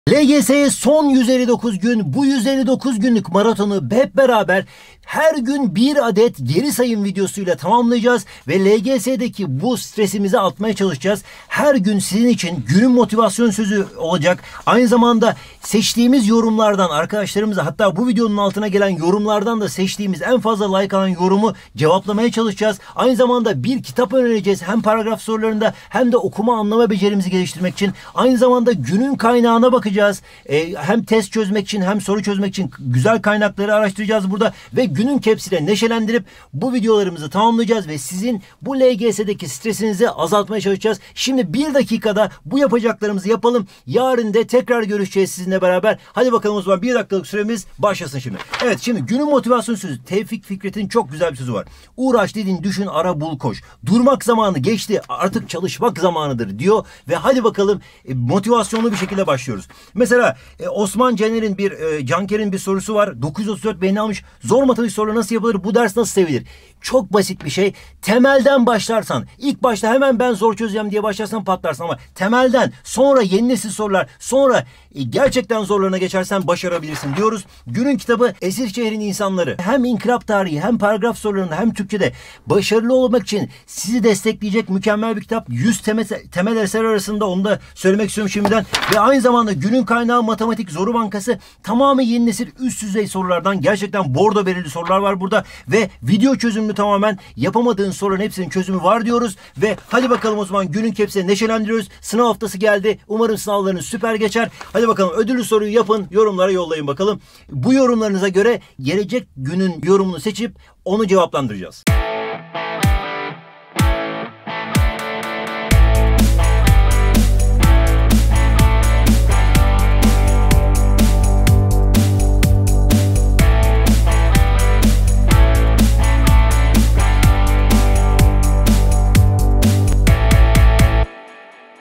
LGS'ye Son 159 Gün! ⏳
LGS'ye son 159 gün kaldı! 📢 LGS'ye geri sayım süreci boyunca sizlere motivasyon olması için her gün bir video ile yanınızdayız!  Ayrıca en fazla like alan yorumu bir sonraki videoda yanıtlandıracağız, bizimle paylaşmayı unutma! 🤓